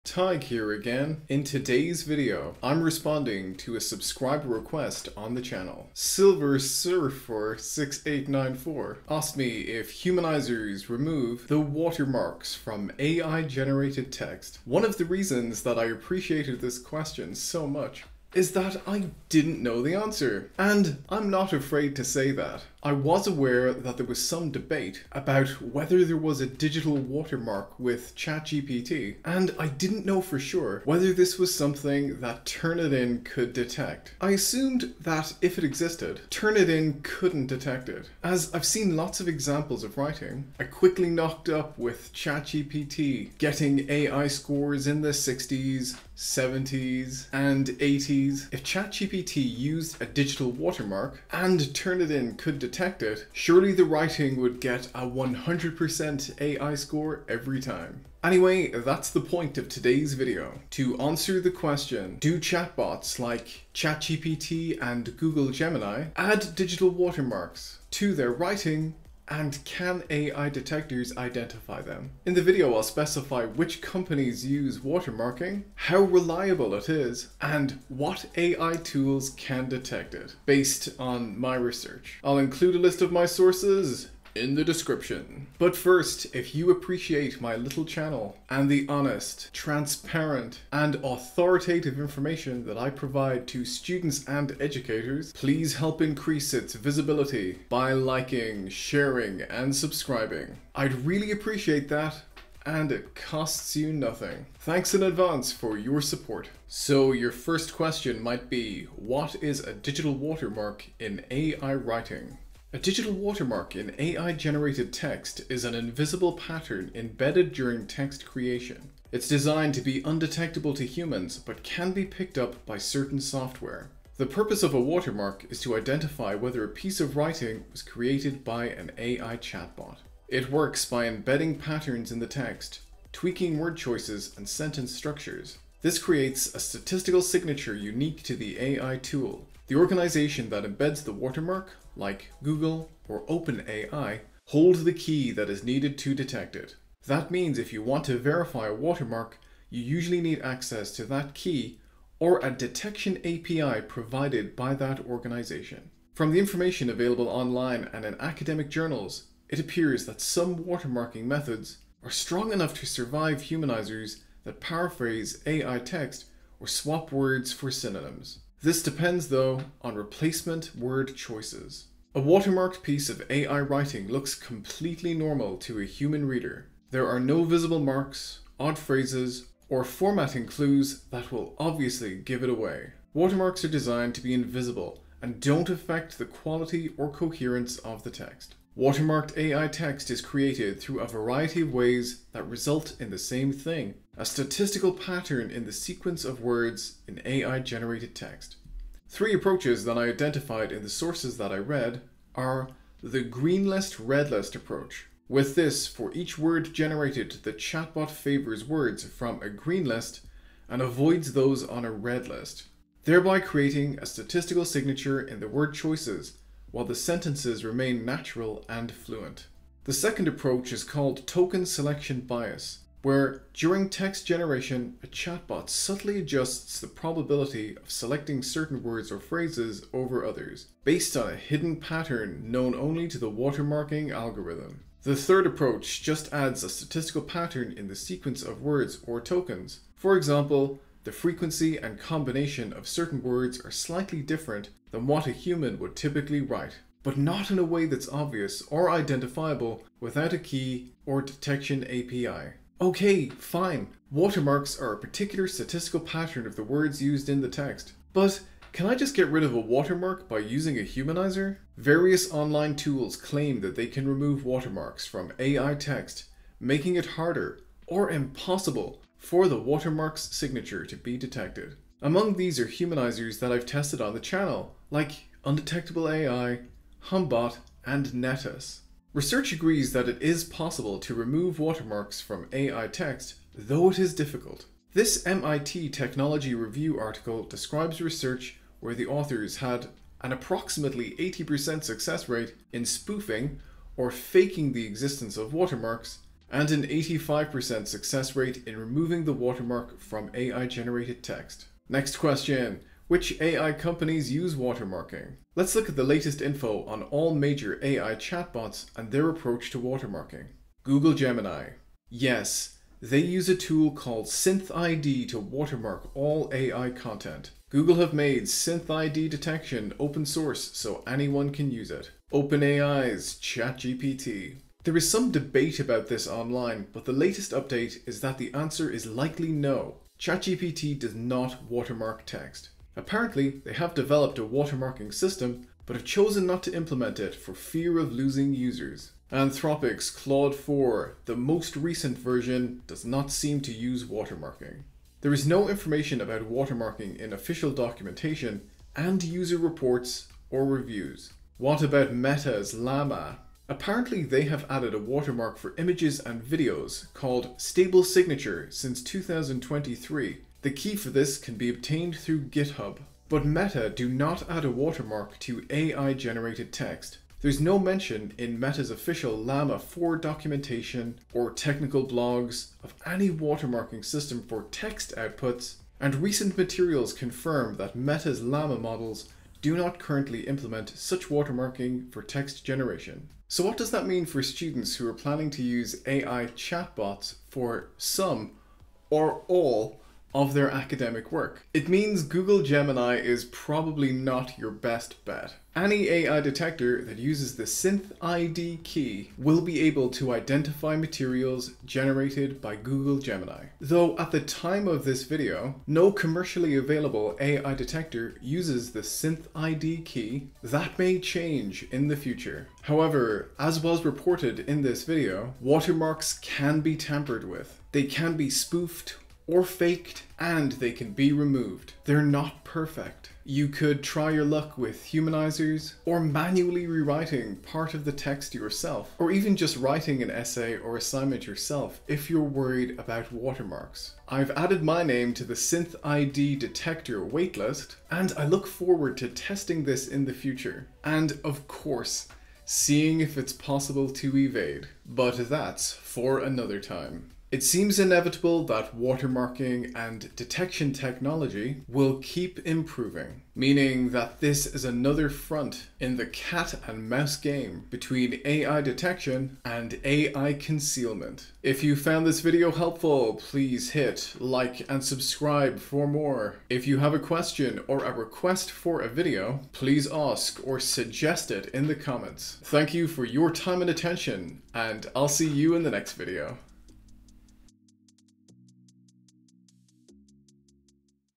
Which AI Chatbot Has Hidden Watermarks Students Should Know About?
Before you use AI for your next assignment, watch this! I investigated whether AI humanizers can actually remove the hidden digital watermarks that some AI chatbots embed in their text. The results might surprise you.
In this video, I answer subscriber Sylversyrfer6894's question about AI watermarks and humanizer tools. Here's what you'll learn:

✅ Which major AI chatbots use digital watermarks (spoiler: Google Gemini does!)
✅ How watermarks actually work in AI-generated text
✅ Why ChatGPT might be "safer" than Google Gemini for students
✅ The 80% success rate research on watermark removal

Key Findings:
• Google Gemini: YES - uses SynthID watermarking
• ChatGPT: NO - doesn't implement watermarks (yet)
• Claude 4: NO - no watermarking detected
• Meta Llama: NO - text watermarking not implemented

Full reference list and sources below!

Lancaster, T. Artificial intelligence, text generation tools and ChatGPT – does digital watermarking offer a solution?. Int J Educ Integr 19, 10 (2023).